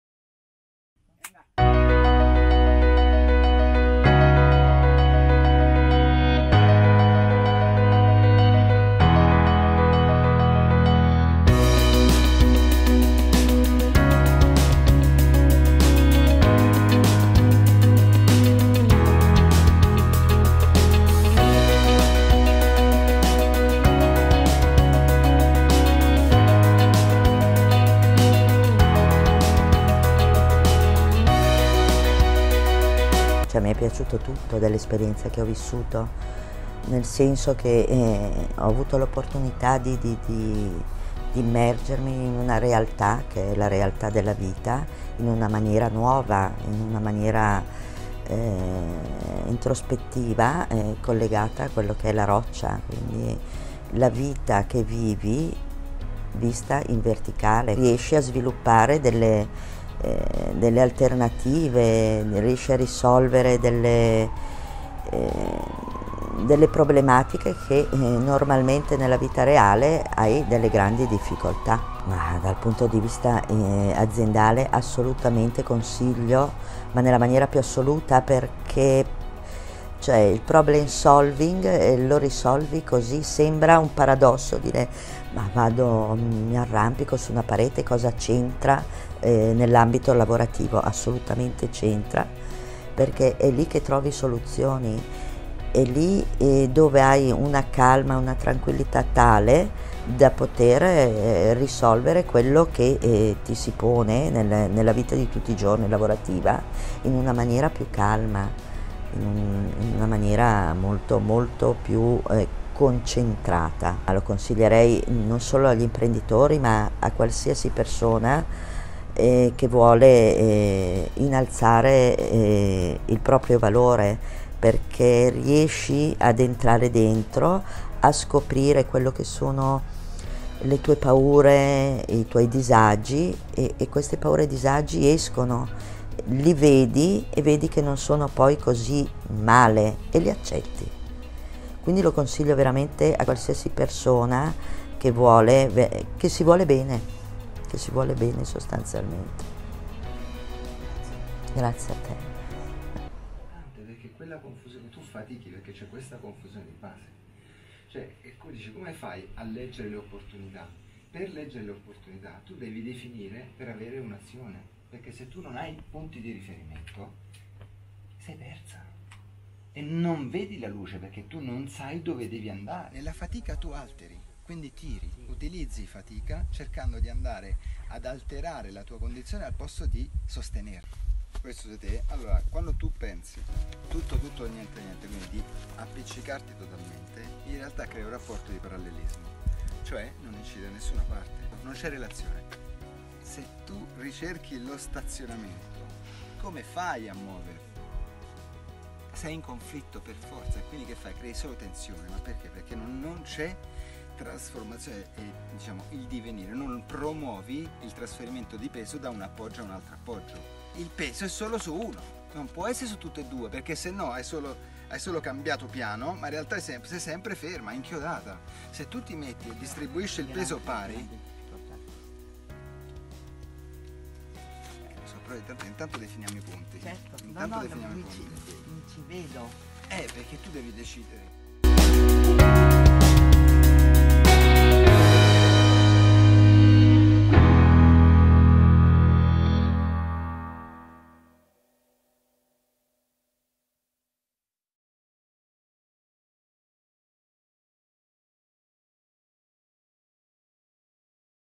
No Limits Camp Andalucia - KataClimb - Busines & Life Adventures
Luglio 2019 - 4 Giorni di Immersione nel Se-Natura
Camp per Imprenditori?!
Si !!

 ✅ KataClimb è un sistema didattico che utilizza
il movimento e l'arrampicata per comprendere comportamenti limitanti e trasformare le vecchie abitudini agganciate alla fatica, lo sforzo ed all'abnegazione..in energia nuova, pulita, fresca 🌀

✅Definisce e fa Vivere i sistemi Sostenibili, Efficienti e Sicuri, come la Natura ed il Movimento in Verticale, secondo una metodologia improntata sul Controllo e sull'Agio😍💪🌀

Grazie a Monica ed Anna per essersi messe in gioco ad un livello così profondo tale da permettersi di impegnarsi in un grande e nuovo progetto di Impresa Efficiente e soprattutto di Vita Gioiosa🙏